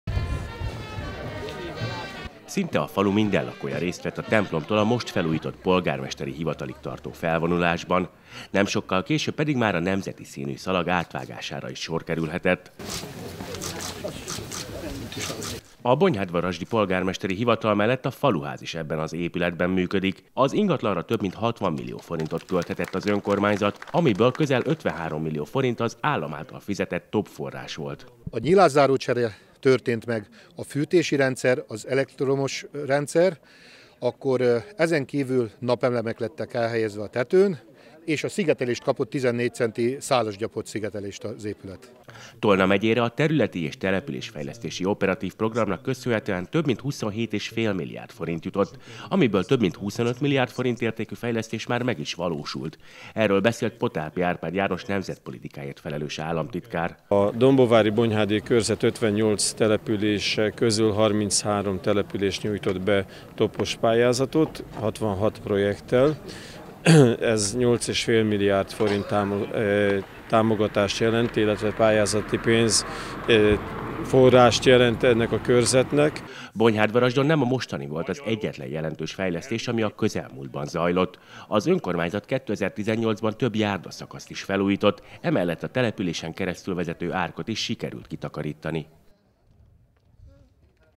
Felújították a bonyhádvarasdi önkormányzat és a helyi faluházat.
60 millió forintból teljesen felújították a bonyhádvarasdi önkormányzat és a helyi faluház által használt épületegyüttest. A pénzből energetikai fejlesztéseket végeztek el: napelemek kerültek a tetőre, szigetelték a falakat, és az elektromos hálózatot is korszerűsítették. Emellett egy hatalmas székely kaput is sikerült a forrásból az ingatlan elé építeni.